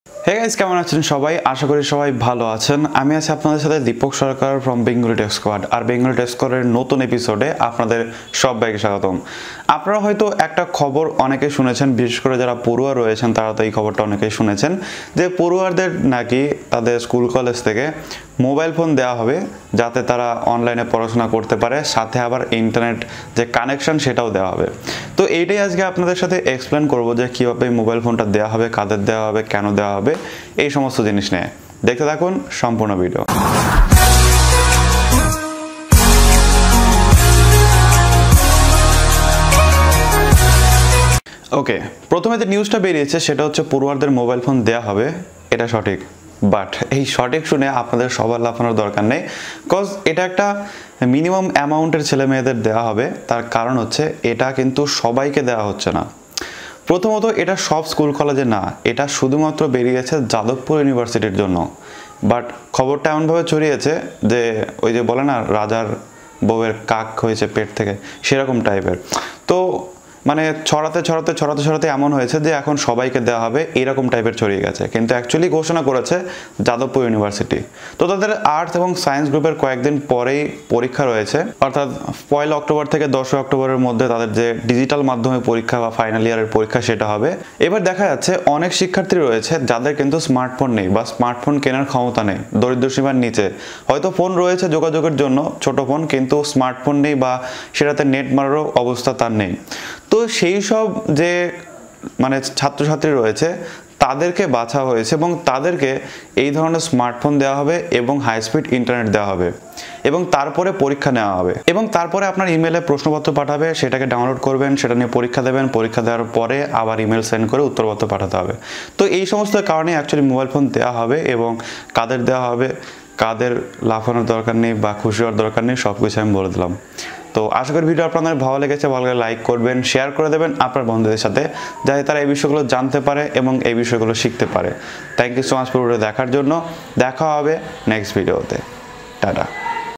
পড়ুয়াদের দেওয়া হবে Smartphone এবং Internet Connection,কারা পাবেন এই Mobile Phone
Bengali Techsquad